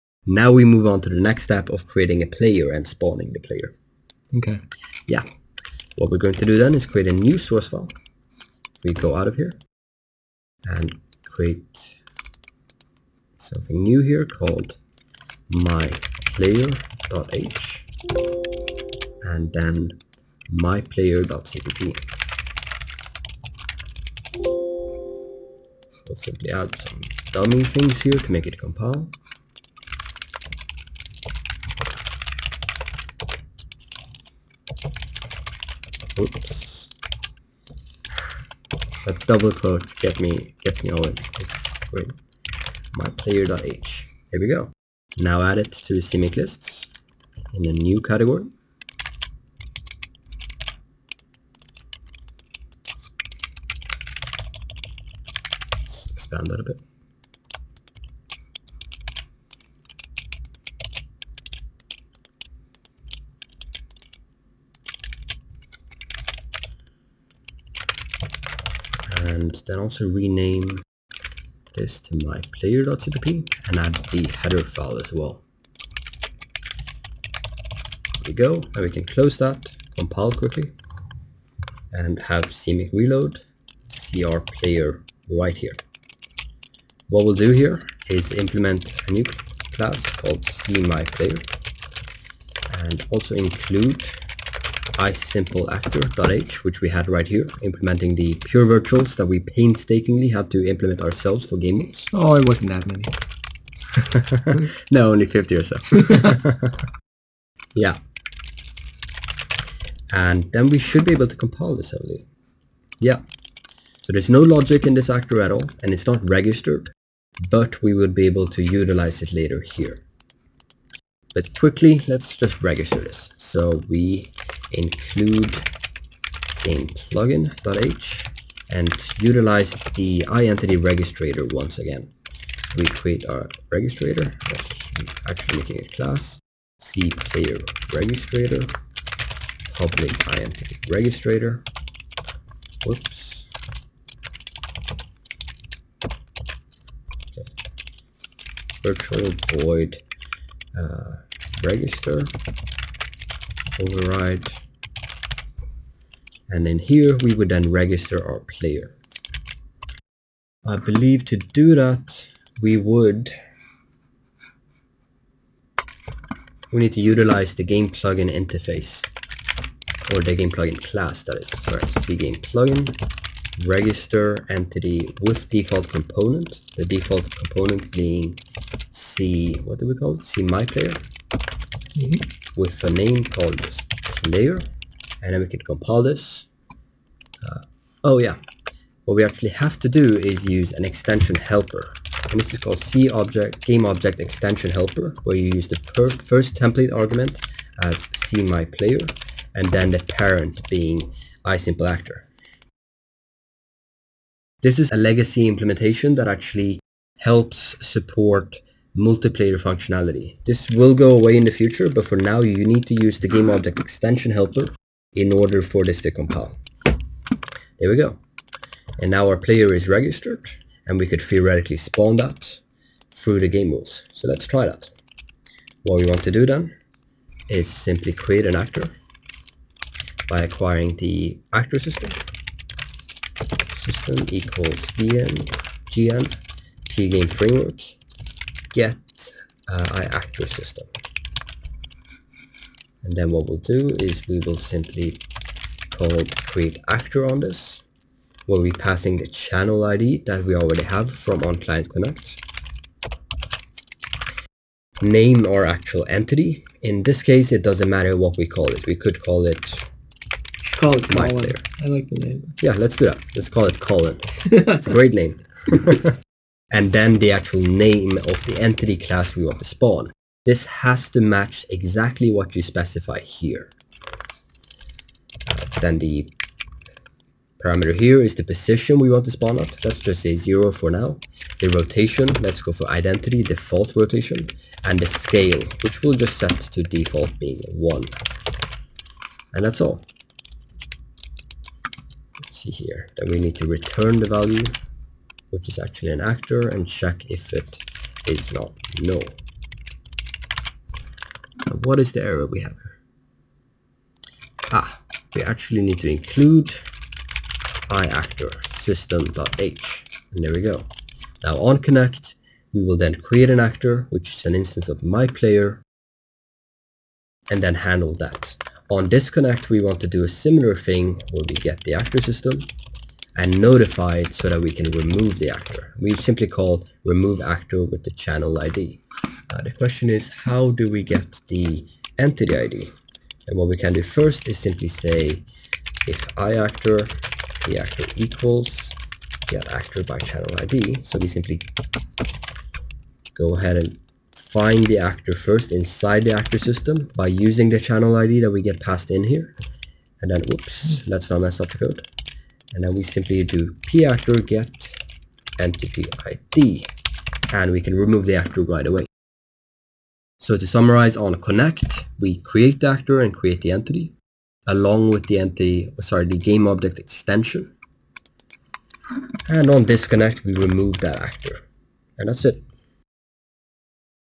How to create a player and gamemode in code [Part 2] | Programming & Projects
In this second part of a three part series we look at how to create a player and then structure the gamemode through the gamerules of the level.